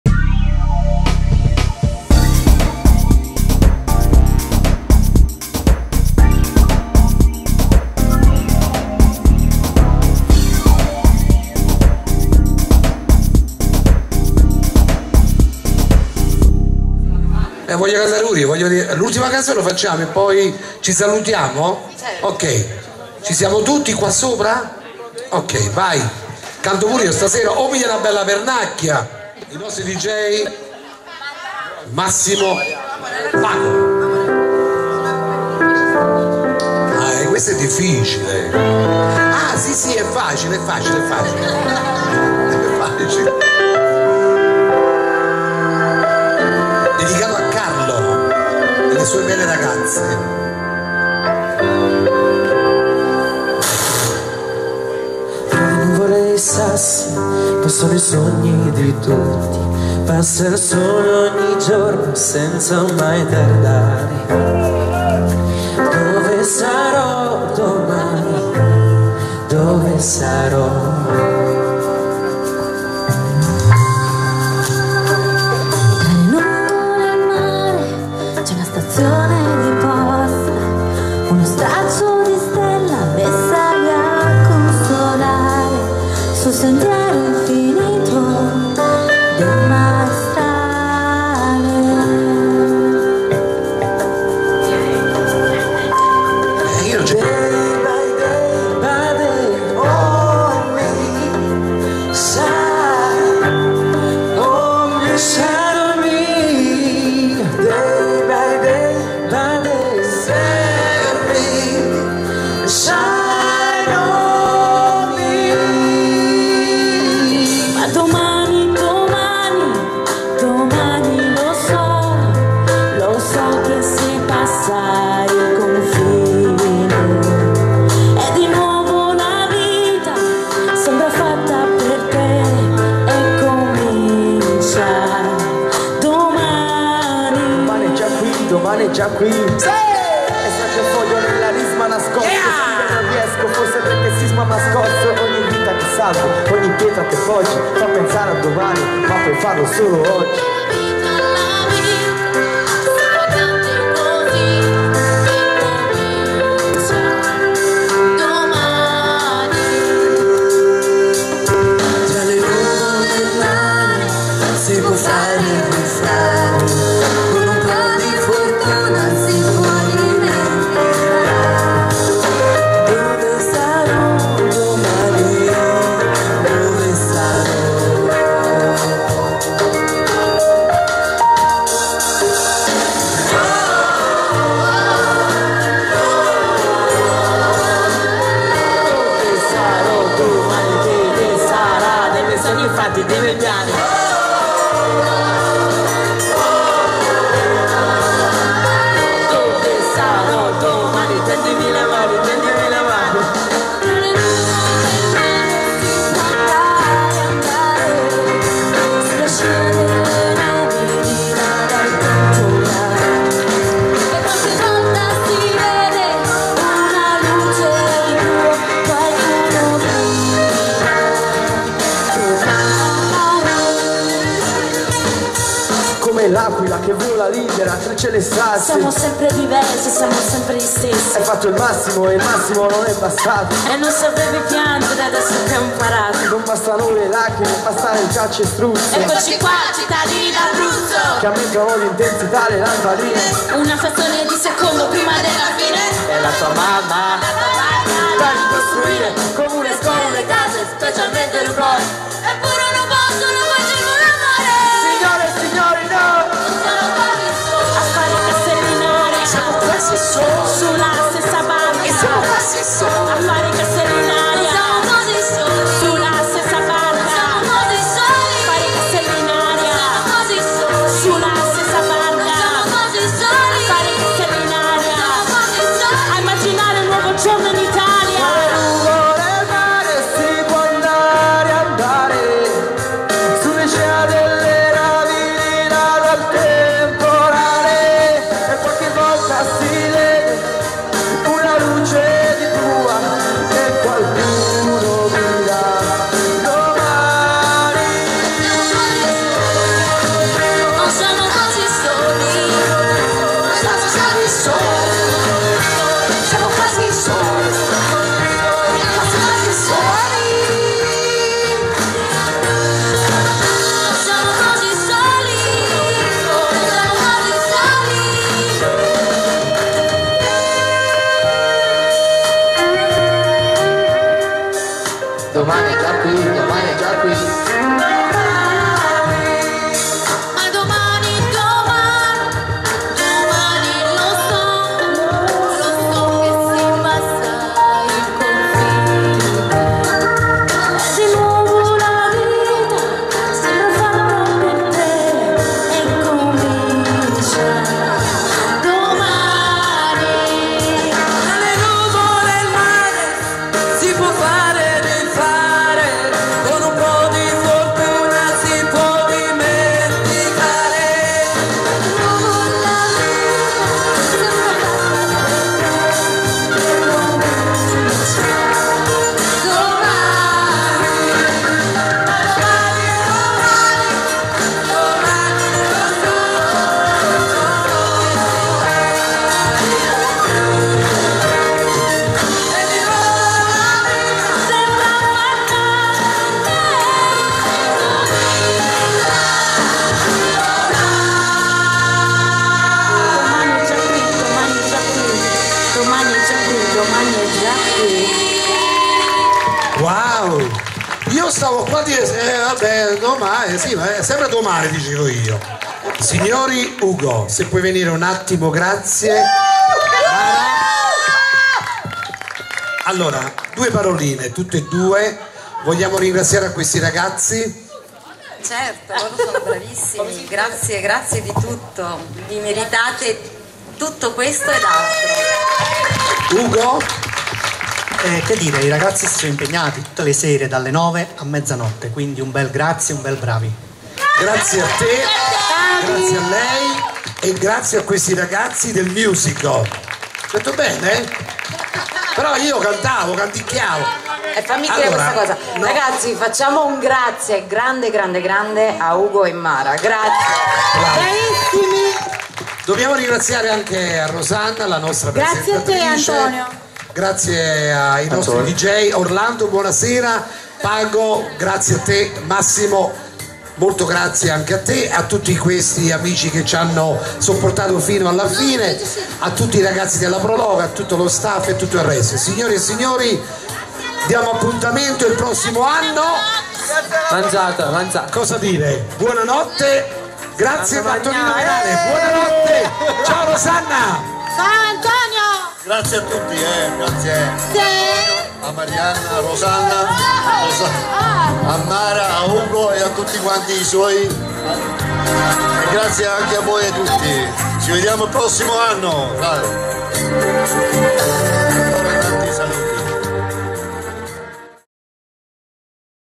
8^ ediz. Festa dell'Uva Fragola e della Mela Annurca -10^ PARTE - 2^ Serata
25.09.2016 TEATRO A CIELO APERTO
Show Musicale con il Gruppo Canoro "CO.ME.TE.

canta: GRUPPO CO.ME.TE.

Saluti finali del presidente della Pro loco ANTONIO MARTINO

SECONDA SERATA - DECIMA PARTE